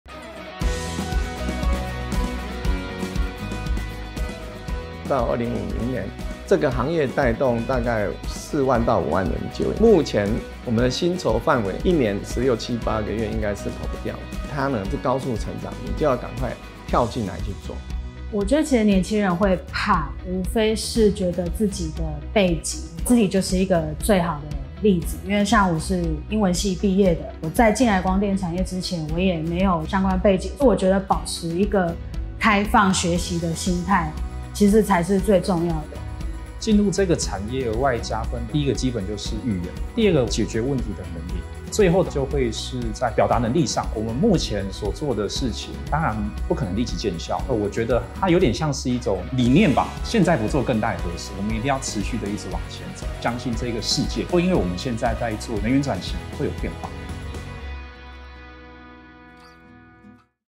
【未來20年最有前景的工作？ 光電產業「綠領」人才大爆發！】
「努力固然重要，但看對未來的方向，比努力更重要！」

太陽光電產業永續發展協會理事長蔡佳晋表示，太陽光電是一項非常跨領域的產業，光職缺就高達20多種。太陽光電產業若順利推展，預估至2050年應該可以帶動4-5萬個就業機會，人才需求十分龐大。

而針對求職者最在意的待遇，蔡佳晋更是驕傲地說：「絕不會讓人失望」，他也鼓勵年輕人多了解並加入光電產業一起努力。